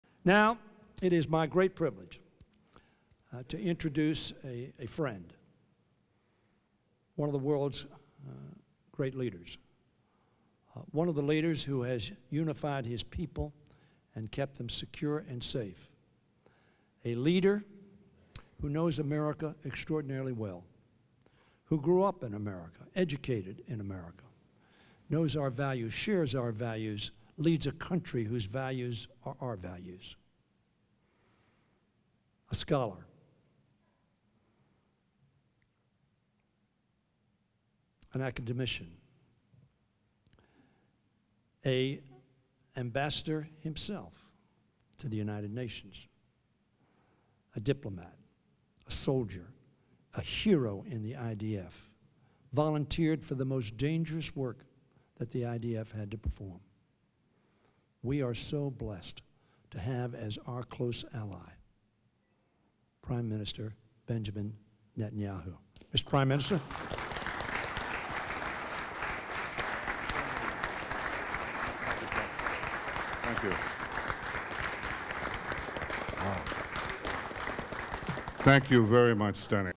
Congressman Steny Hoyer introduces PM Netanyahu
Congressman Steny Hoyer introduces Prime Minister Netanyahu at farewell event for outgoing Israeli Ambassador to the US Michael Oren.